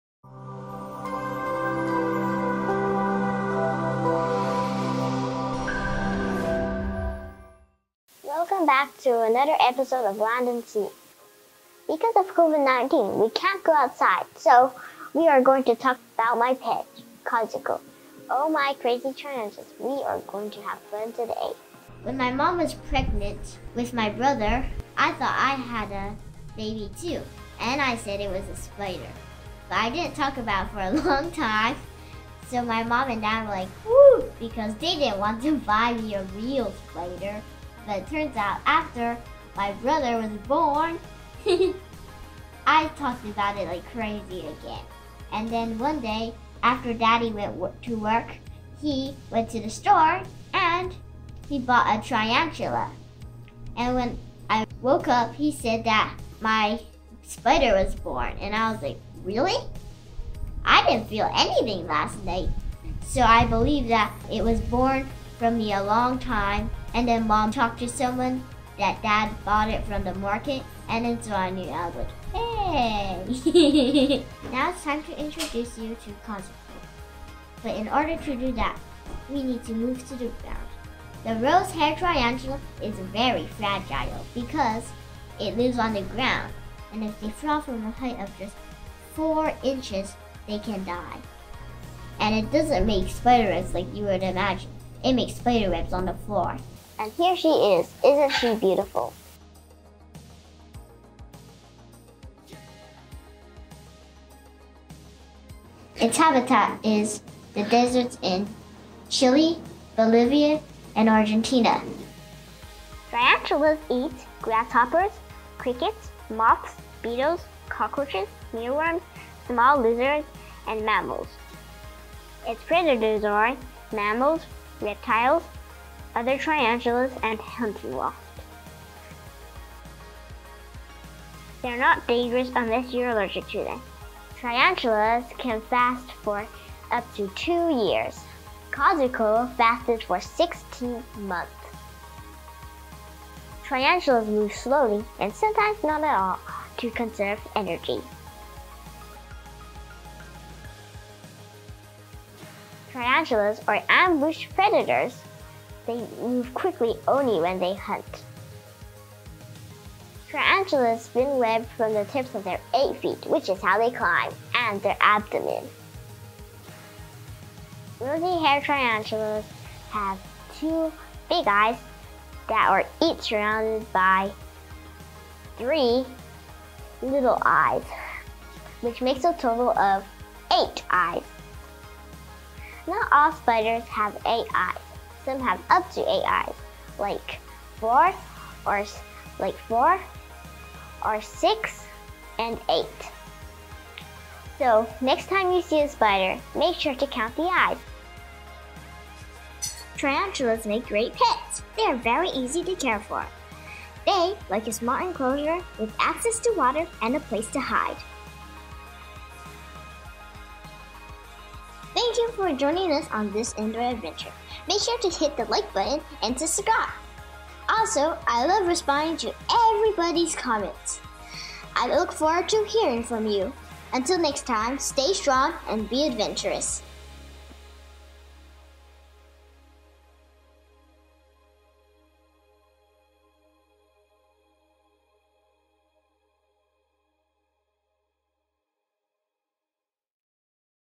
My Amazing Pet Spider- All about Rosy Tarantula
Since COVID-19 has us in lockdown we stay indoors we can't brave the wilderness. In this episode of Land & Sea, I introduce you to my pet! It is probably not what you think it is It's a tarantula and definitely not a pet every 9-year-old girl would want and I have had it since I was 4! Come and listen to my story of why I have it, you'll love it. It is going to be lots of fun! I would love to hear!

Learn about tarantulas and have some fun while doing it.